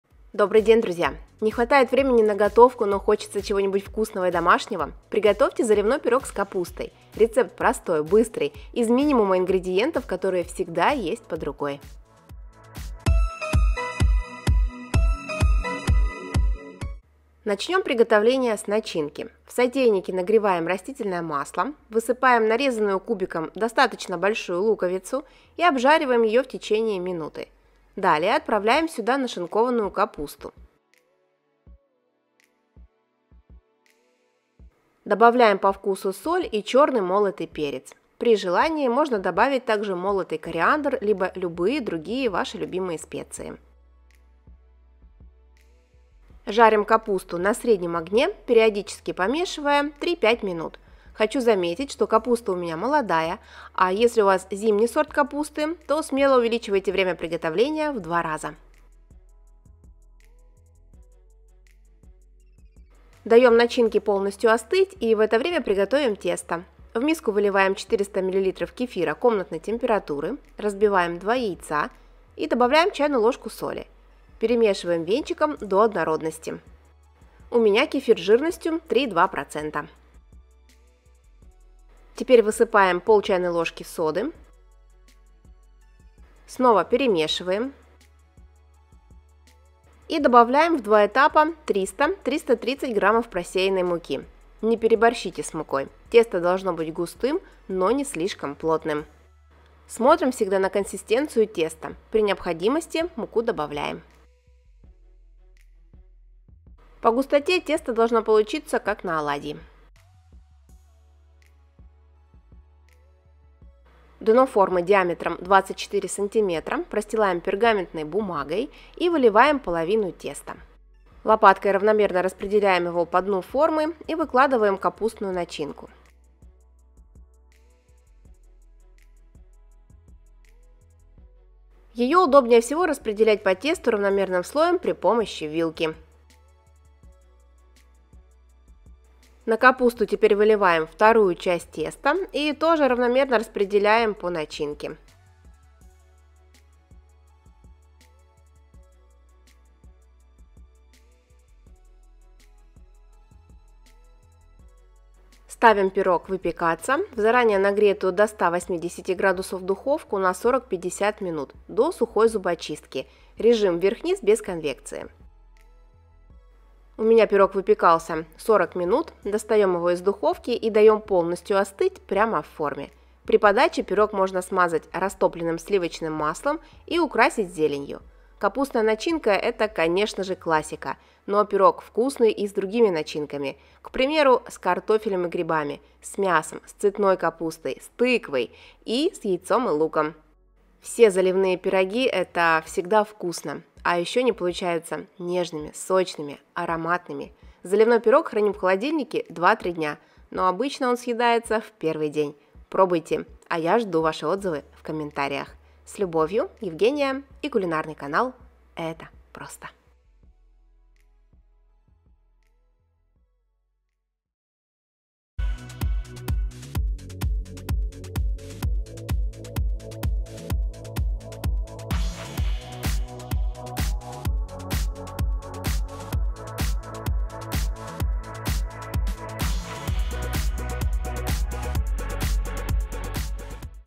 Заливной пирог на кефире с капустой: самый простой, быстрый и вкусный рецепт
Не хватает времени на готовку, но хочется чего-нибудь вкусного и домашнего? Приготовьте заливной пирог с капустой. Рецепт простой, быстрый, из минимума ингредиентов, которые всегда есть под рукой!

ИНГРЕДИЕНТЫ:
Тесто:
кефир (3,2%) - 400 мл

Выпекаем при 180°С 40-50 минут. Режим: верх-низ, без конвекции.
✅ Друзья, мои КНИГИ рецептов "Это просто Выпечка"! и "Выпечка с любовью" можно приобрести на Ozon, Wildberries, Читай-город, Лабиринт, Буквоед и Бук24